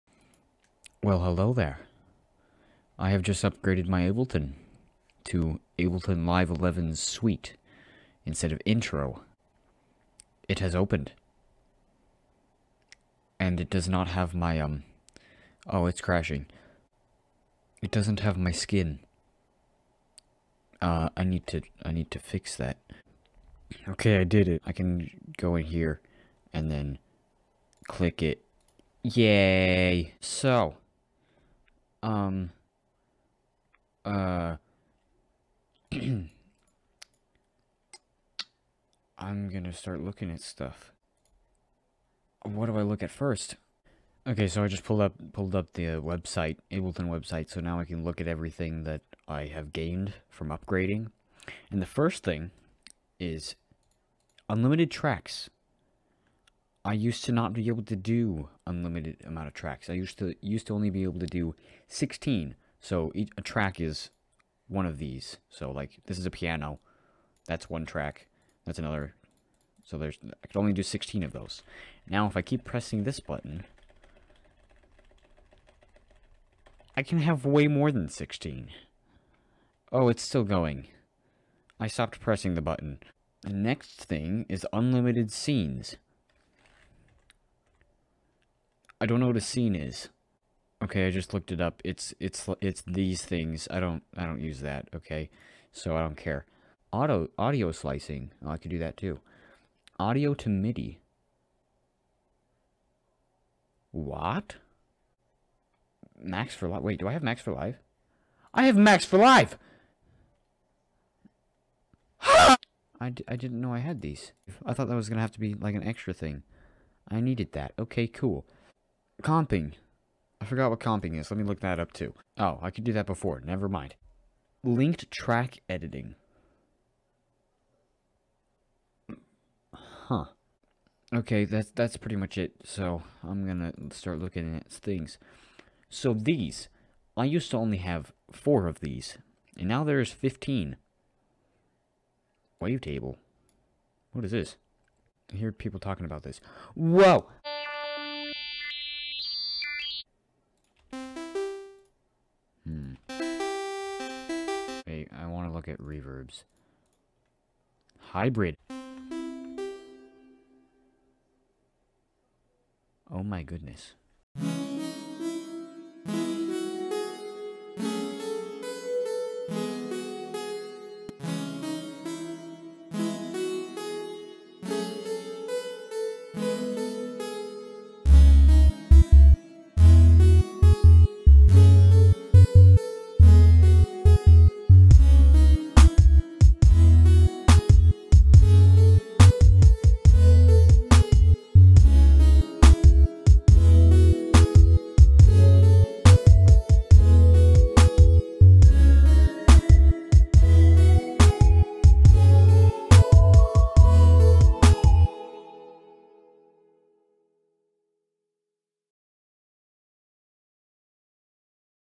My first look at Ableton Live 11 Suite
I Got Ableton Live 11 Suite as an early Christmas present, here's my first look at it!